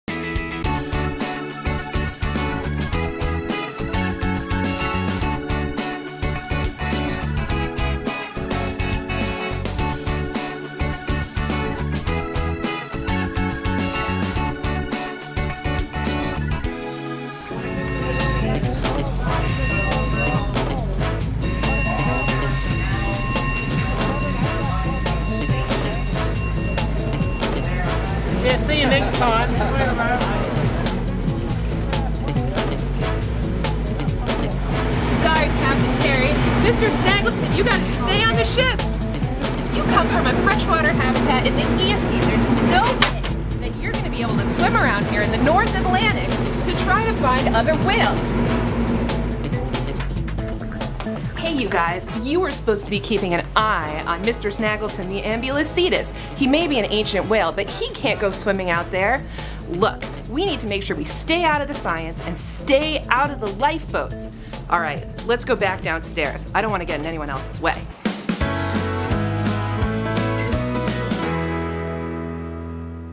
Eocene Invasion: Ambulocetus
Mr. Snaggleton the Ambulocetus is trying to escape in one of the life boats! Check out the trouble that this Eocene cousin of modern day whales is getting into on the JOIDES Resolution...